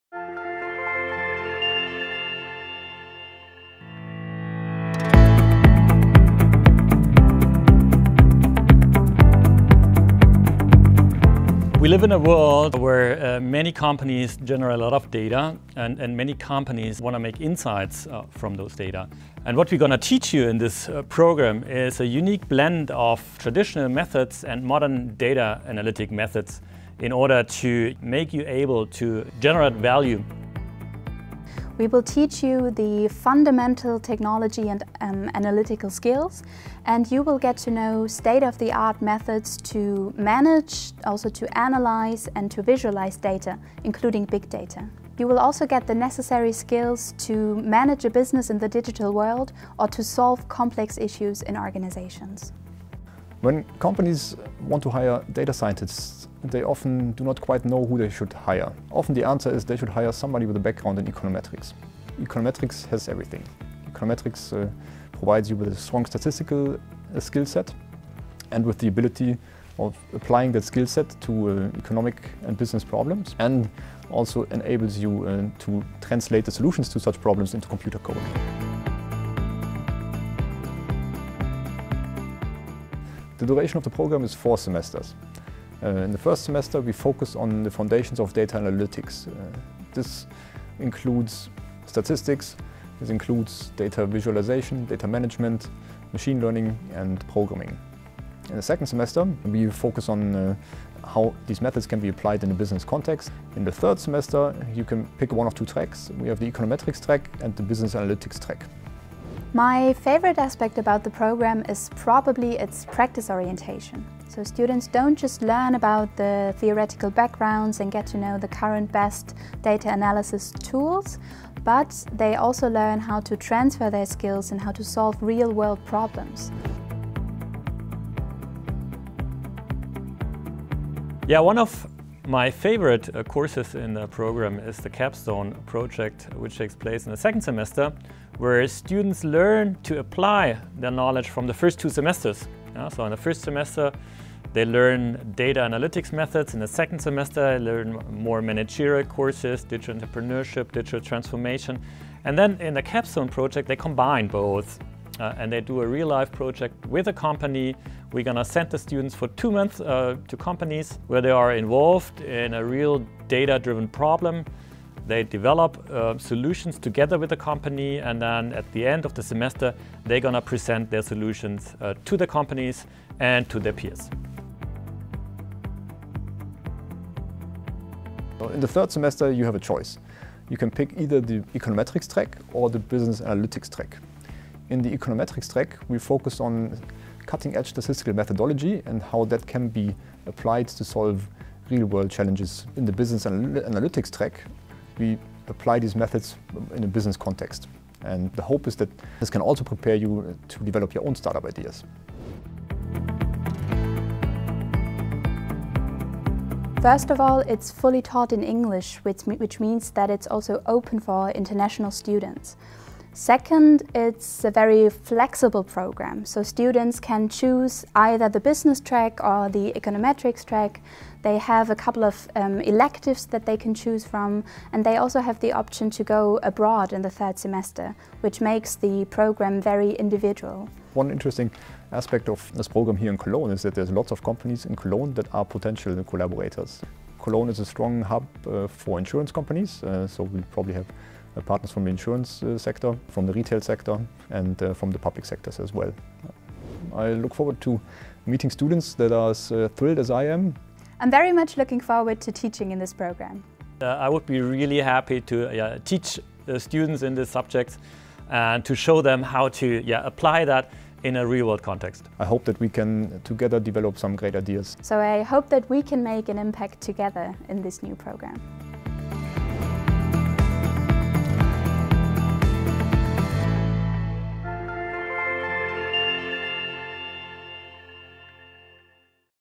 Master in Business Analytics & Econometrics - From Data To Insights
Data analytics, digital innovation and big data are crucial developments for the upcoming challenges in all business and economic areas. This new programme here at the wiso faculty offers a unique blend of traditional analytics (e.g. statistics, econometrics, optimisation, simulation) and modern analytical approaches (e.g. machine learning and artificial intelligence using R and Python) and merges these approaches with the teaching of groundbreaking knowledge in the areas of economics, management, and entrepreneurship.